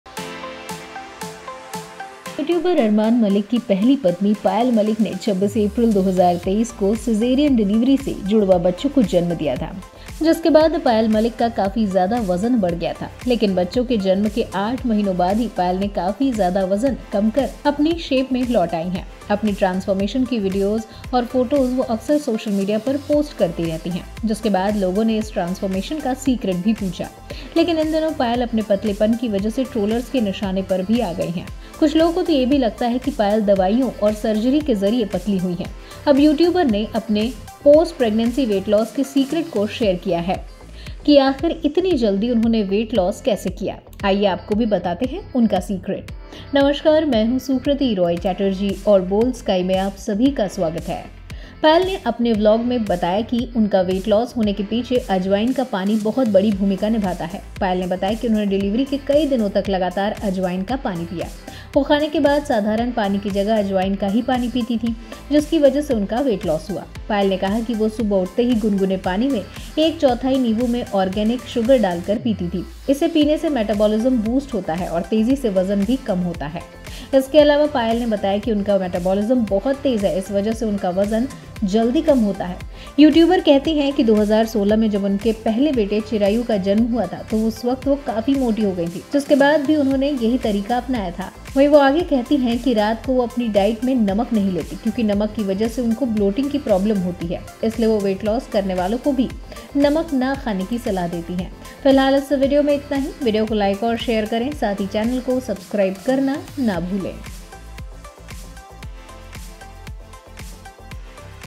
Payal Malik Weight Loss Journey: Payal Malik Weight kaise Kam Kiya | Boldsky
Youtuber Armaan Malik Wife Payal Malik : यूट्यूबर अरमान मल‍िक की पहली पत्‍नी पायल मल‍िक ने 26 अप्रैल 2023 को सीजेरियन डिलीवरी से जुड़वा बच्‍चों को जन्‍म दिया था। जिसके बाद पायल का वजन काफी ज्‍यादा बढ़ गया था।लेक‍िन बच्‍चों के जन्‍म के 8 महीनों बाद पायल ने काफी वजन कम करके शेप में आ गई थी। अपनी ट्रांसफॉर्मेशन की फोटो पायल ने सोशल मीडिया में भी शेयर की थी। ज‍िसके बाद लोगों ने उनसे इस ट्रांसफॉर्मेशन का सीक्रेट भी पूछा था। लेक‍िन इन दिनों पायल अपने पतलेपन की वजह से ट्रोलर्स के न‍िशाने पर आ गई हैं।

Youtuber Armaan Malik Wife Payal Malik: Youtuber Armaan Malik's first wife Payal Malik gave birth to twins through cesarean delivery on 26 April 2023. After which Payal's weight had increased a lot.But 8 months after the birth of her children, Payal had lost a lot of weight and got in shape. Payal also shared the photo of her transformation on social media. After which people also asked him the secret of this transformation. But these days Payal has become the target of trolls because of her thinness.